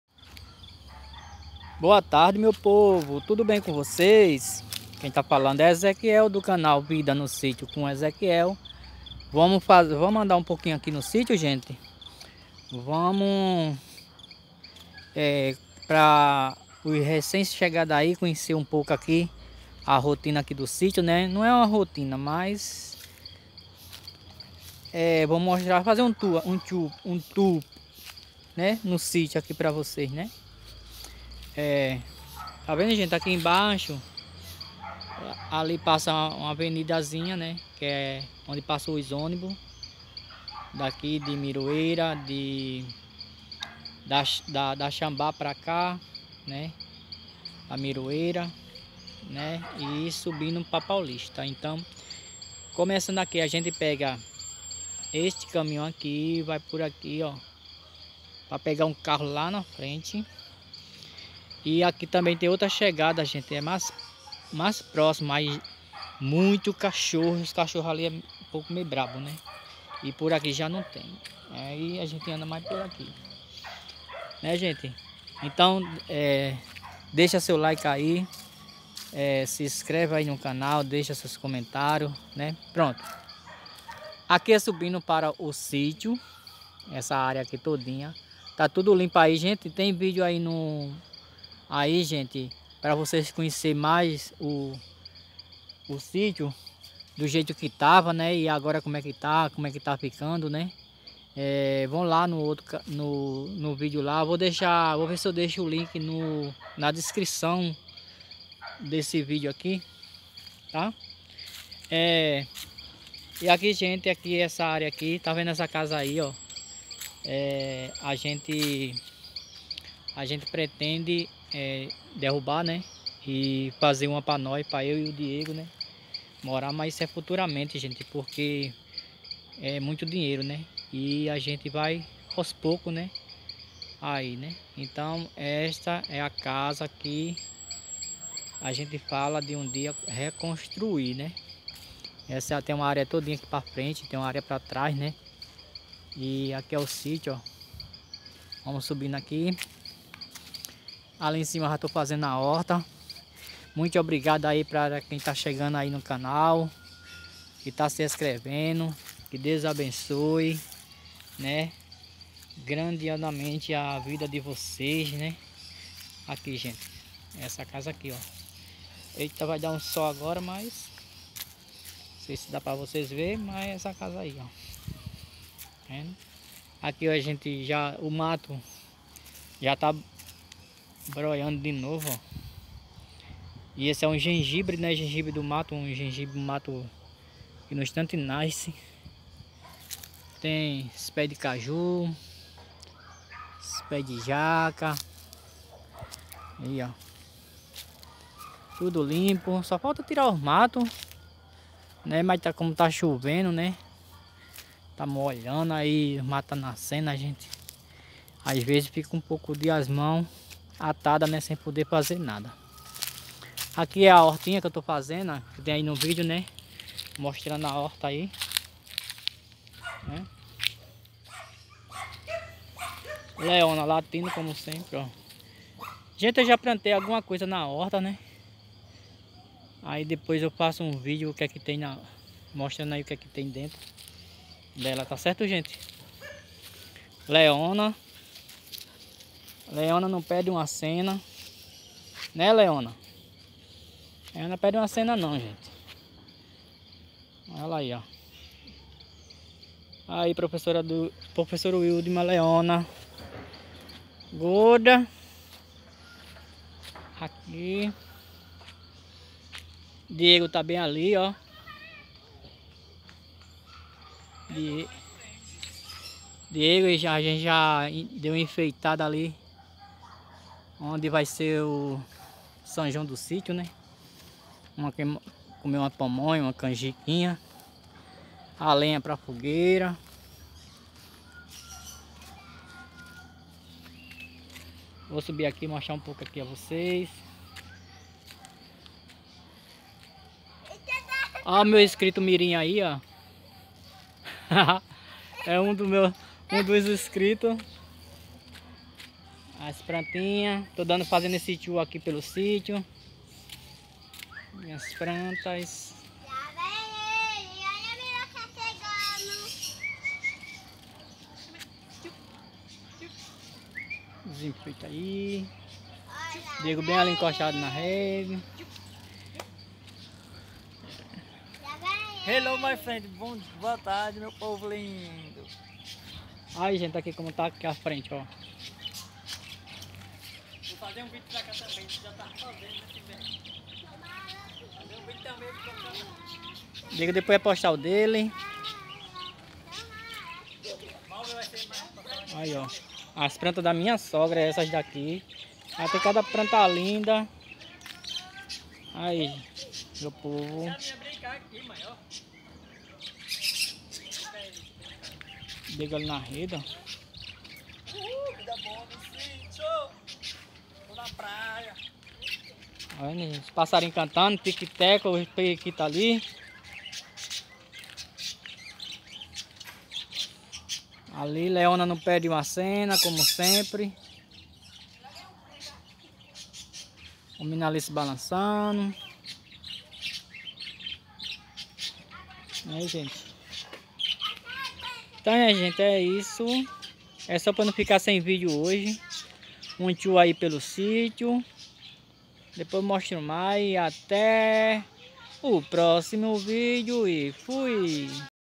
tuor pelo sítio novo Onde nós estamos orando
Oi gente boa tarde Este é mais um vídeo para vocês assistirem dúvida no sítio com Ezequiel espero que vocês gostem muito pelo novo sítio Onde nós estamos morando